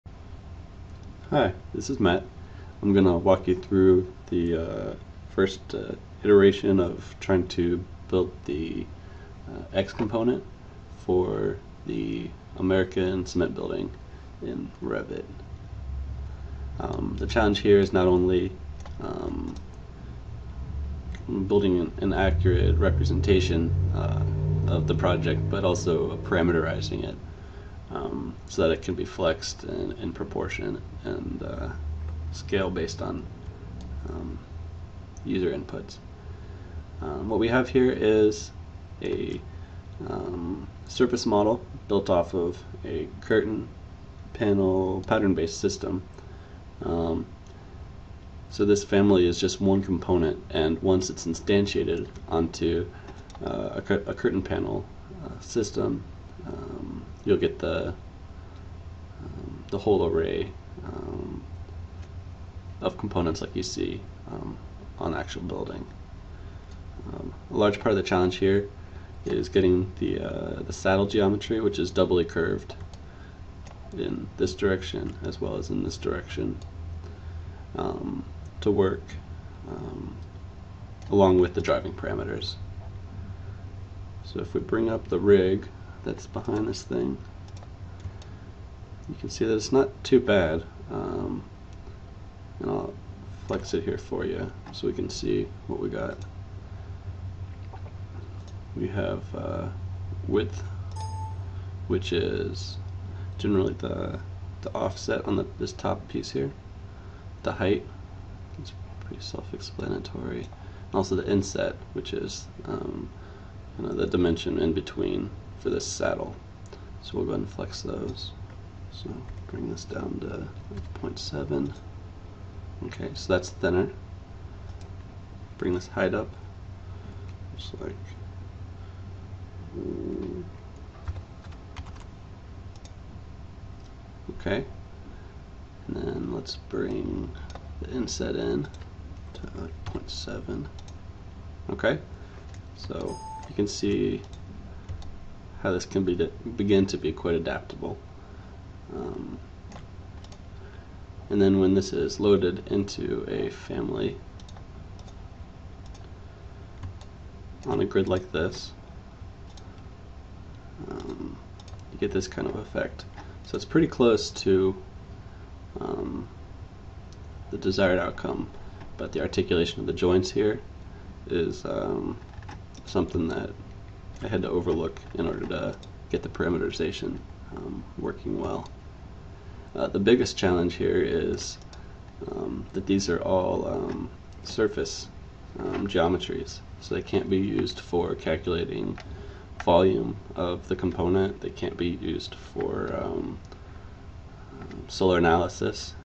American Cement Component Revit 2011
a walkthrough of a parameterized component construction.